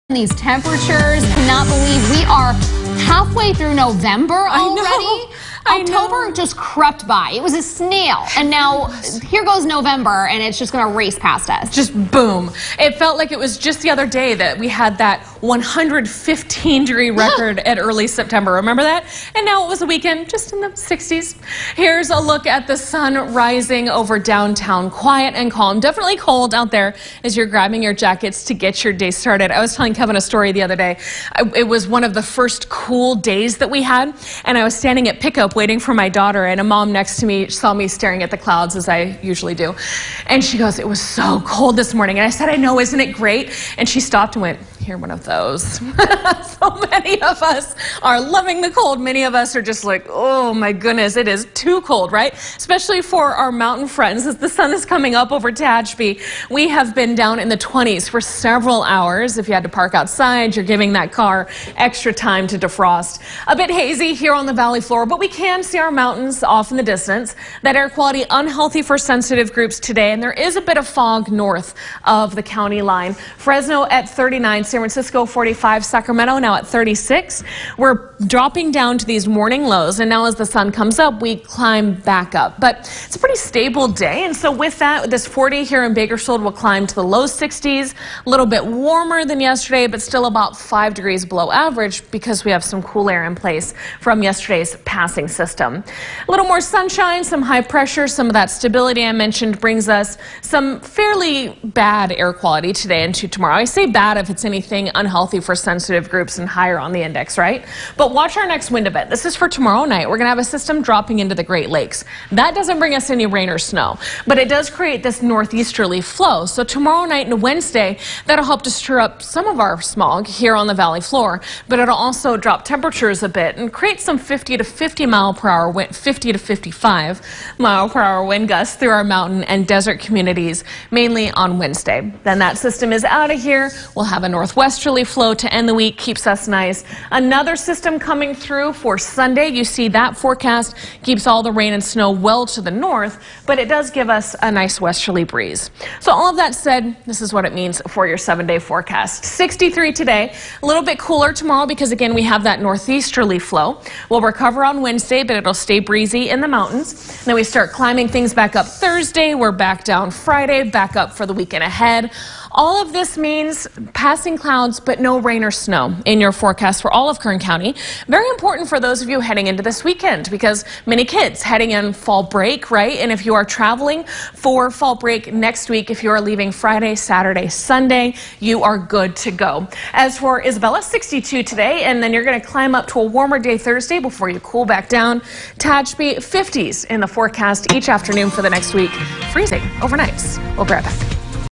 Dry weather, brisk temperatures expected throughout Kern County
Dry weather will prevail through the start of next week, with temperatures continuing to run below normal for this time of year. Outside of urban areas, near freezing temperatures are expected in the San Joaquin Valley tonight through Wednesday night. Gusty winds will develop in mountain and desert areas Tuesday evening and Wednesday.